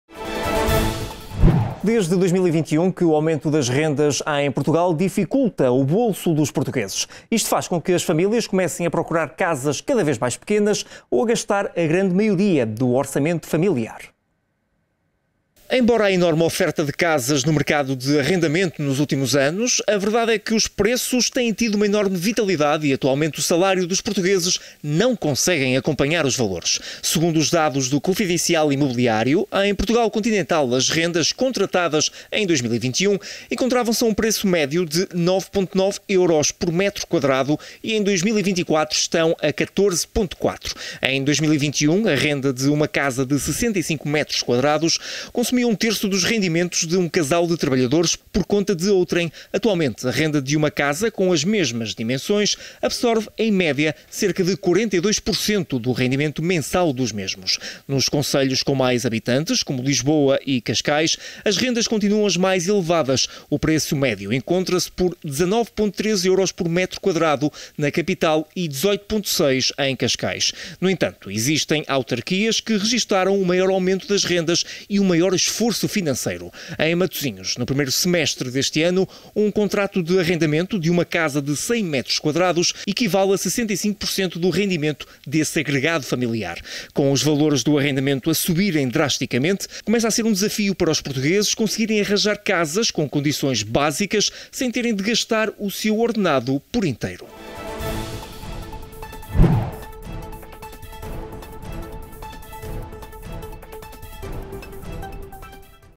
Rendas das casas estão cada vez mais altas
Desde 2021 que o aumento das rendas em Portugal dificulta o bolso dos portugueses. Isto faz com que as famílias comecem a procurar casas cada vez mais pequenas ou a gastar a grande maioria do orçamento familiar.